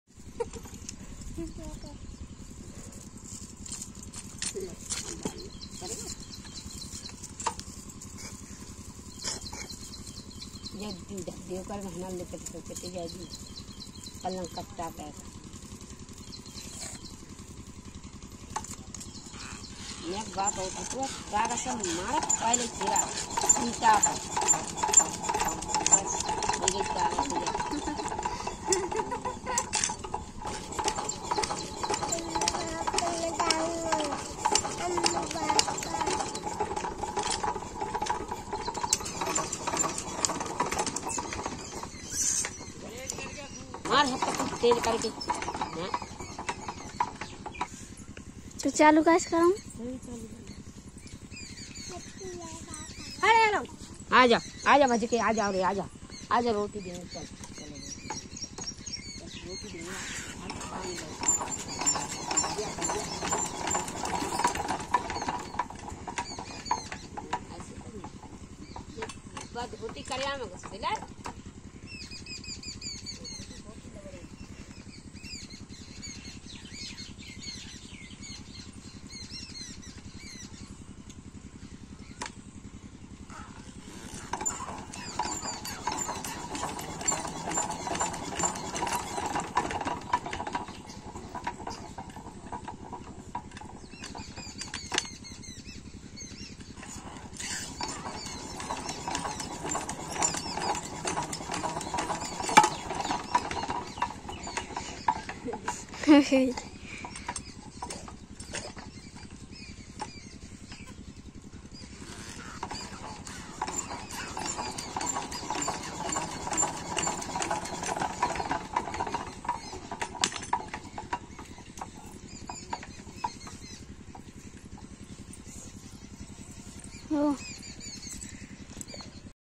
Diesel engine l Water pumping machine l गांव की महिला द्वारा डीजल इंजन water pump सेट मशीन स्टार्ट
Diesel engine l Water pumping machine set l गांव की महिला द्वारा डीजल इंजन water pump सेट मशीन स्टार्ट#dieselengine